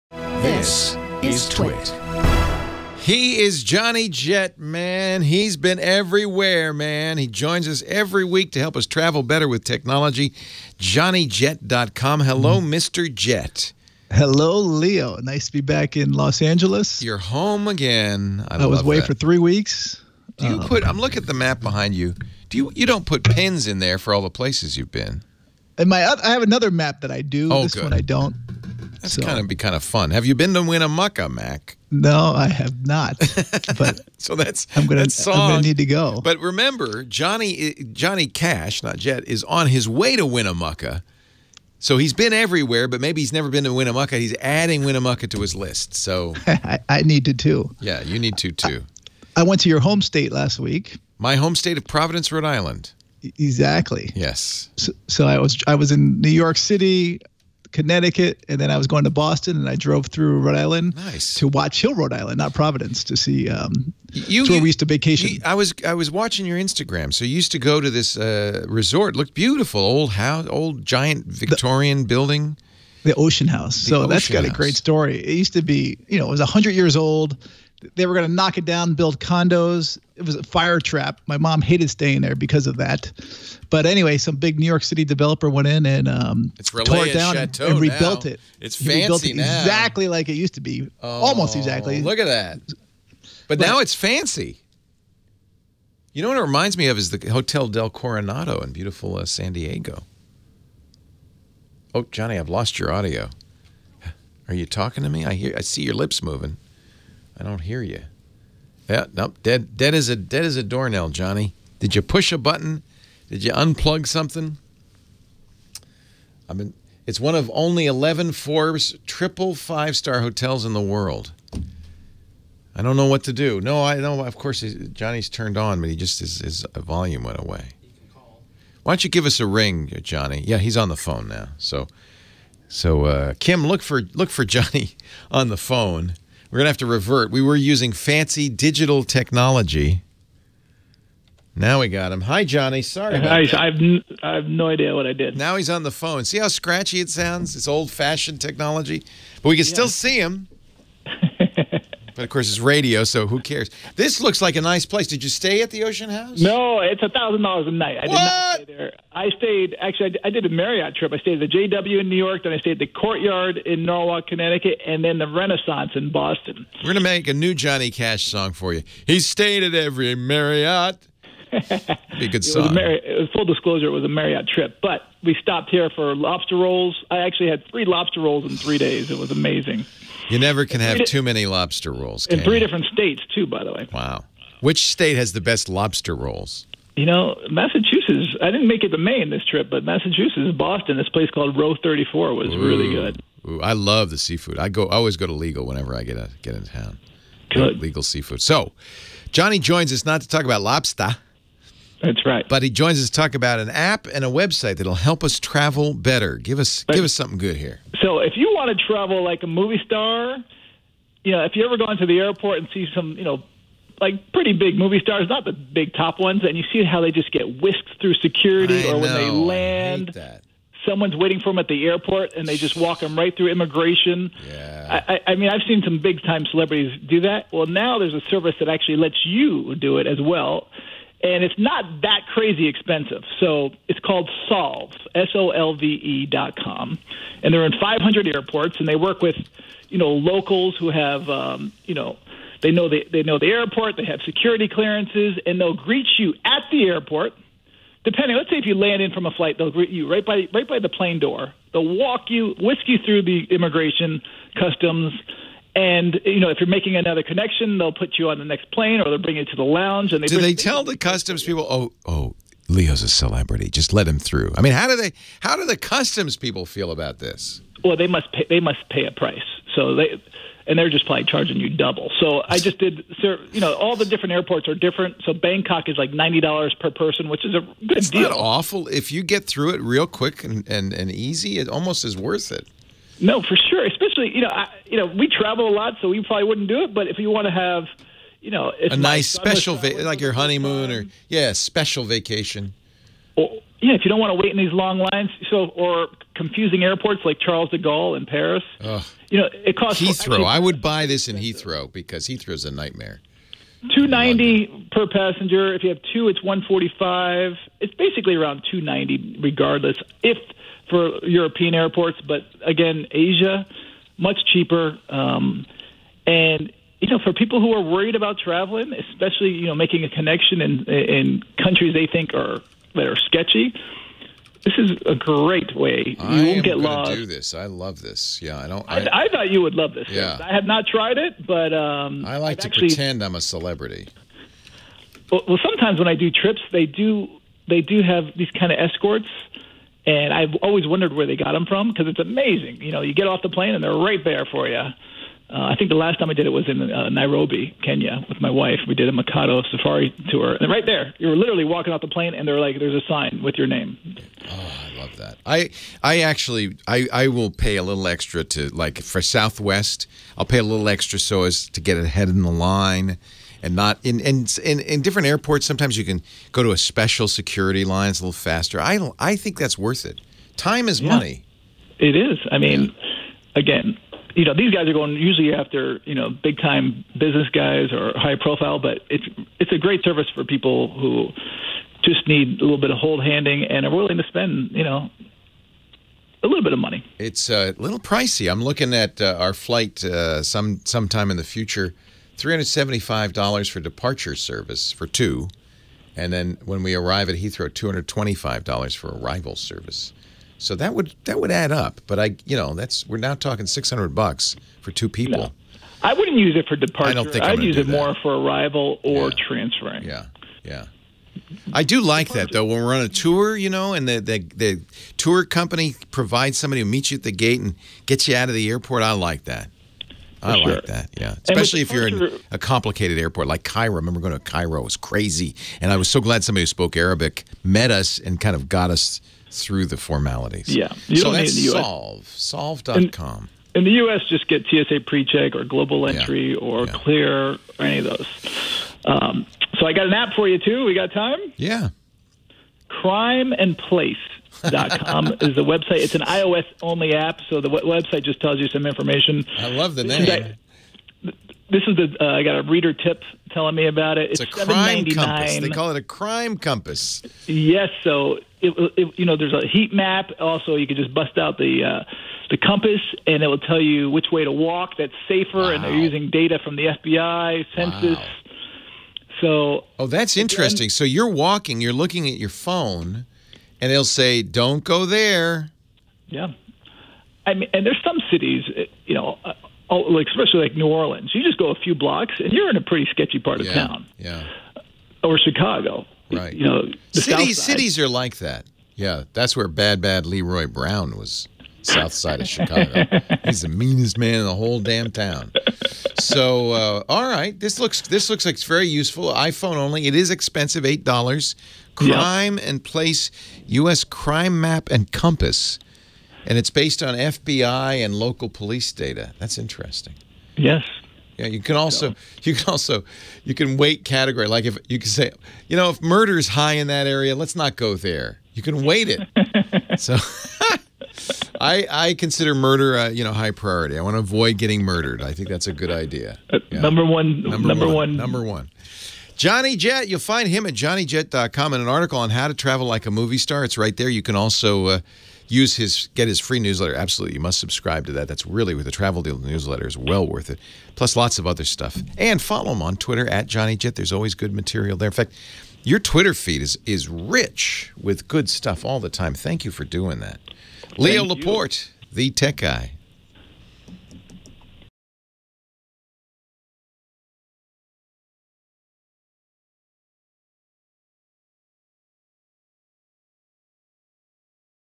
Get Through Immigration and Customs Fast with Solve.com
Johnny Jet and Leo Laporte talk about Solve.com, a service to get you through immigration and customs faster when traveling, and Crime and Place, an app to see crime levels in your surrounding area.